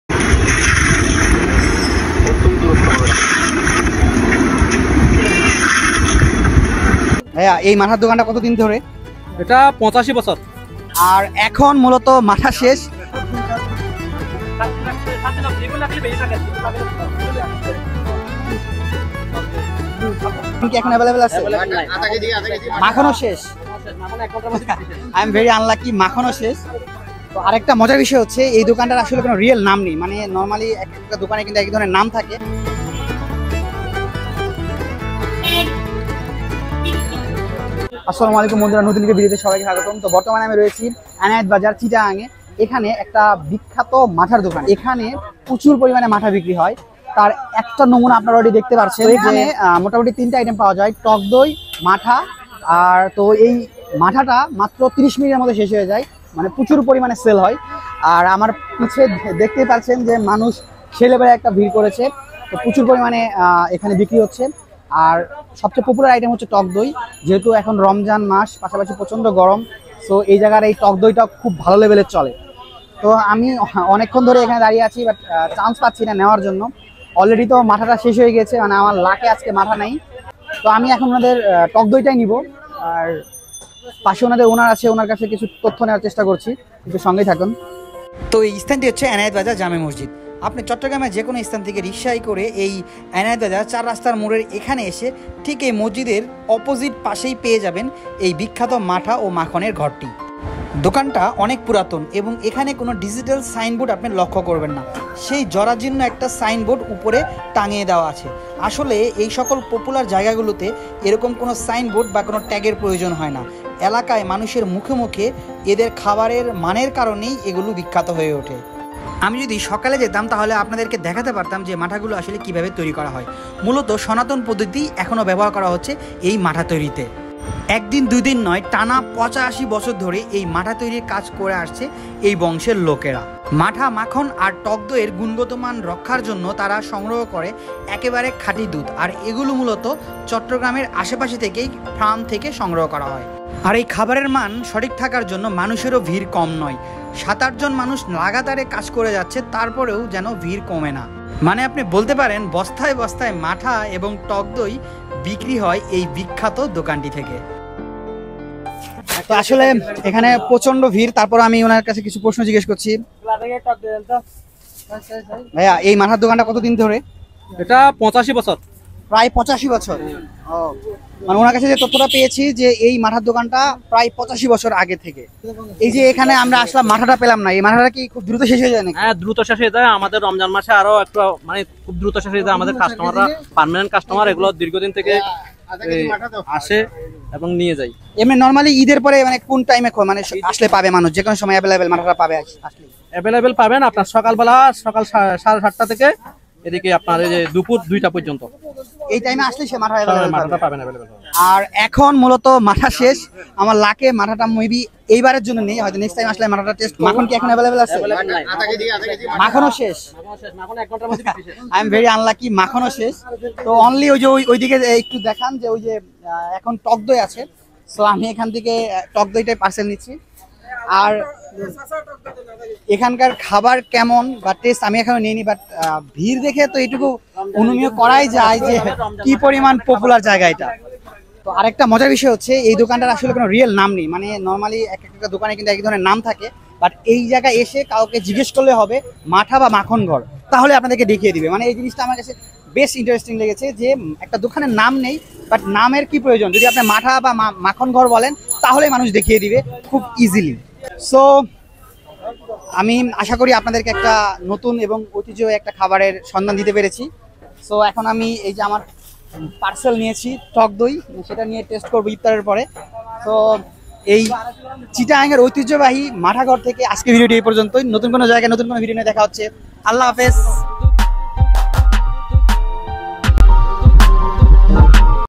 ৮৫ বছরের পুরনো মাঠা আর মাখন||এনায়েত বাজার,চট্রগ্রাম
এই দোকানটির নির্দিষ্ট কোন নাম নেই||এনায়েত বাজারে গিয়ে মাঠা বা মাখন ঘর বললেই মানুষ দেখিয়ে দেয়||প্রায় ৮৫ বছরের পুরনো ঐতিহ্যবাহী মাঠা আর মাখন!